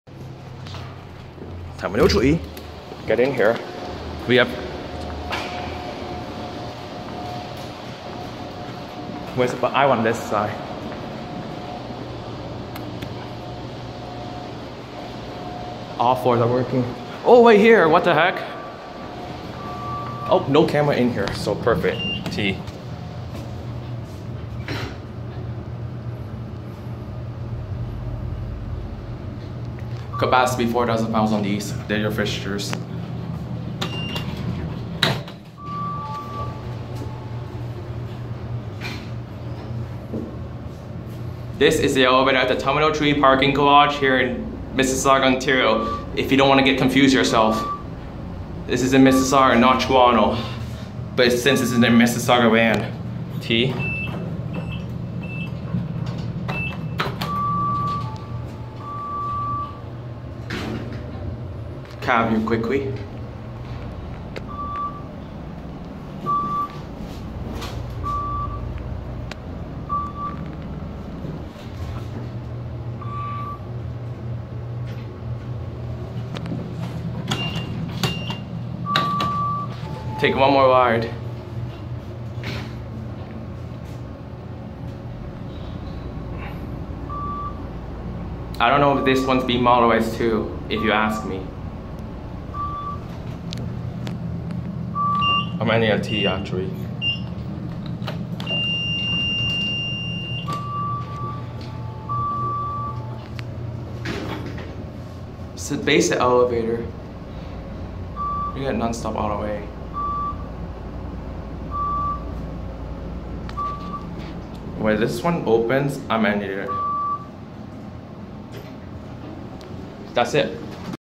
ThyssenKrupp Northern Traction Elevators at Toronto Pearson Airport Terminal 3
Here is the third and final set of Terminal 3 Parking Garage Elevator at Toronto Pearson Airport in Mississauga ON. This bank, all 4 elevators are in service not like other 2 banks. This set is added it on later on while they did their expansion and include the tunnel level to the terminal. The elevator are nice but basic.